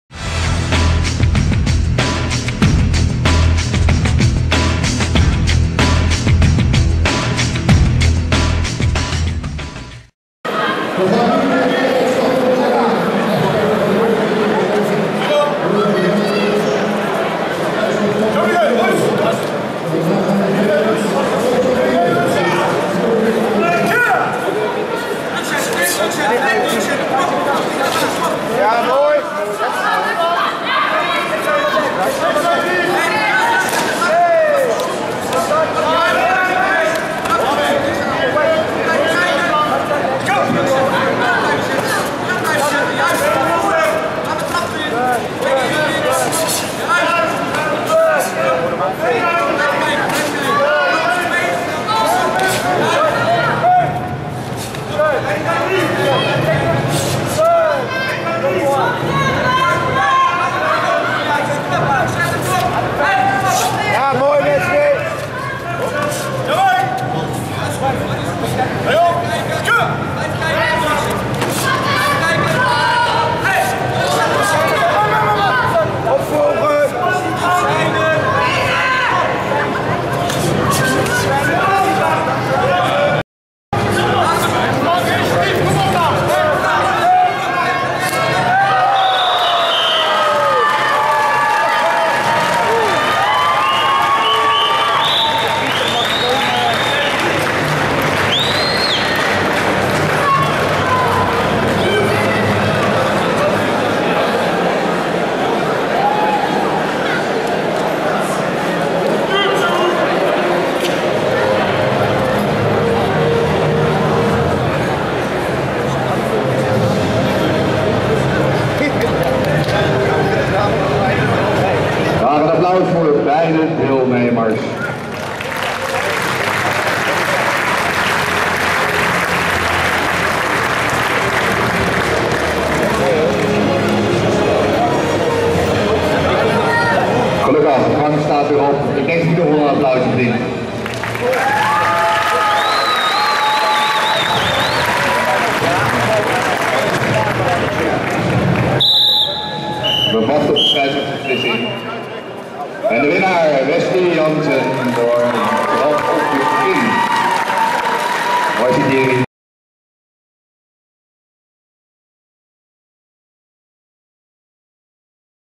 2016 05 21 NK Kyokushin mat 2 fight 14
Voorronde gevecht op het Nederlands Kampioenschap Kyokushin Karate 2016 in Ahoy, Rotterdam - MAT 2.
Prelim fight Dutch Championships Kyokushin Full Contact Karate 2016 @ Ahoy, Rotterdam - MAT 2.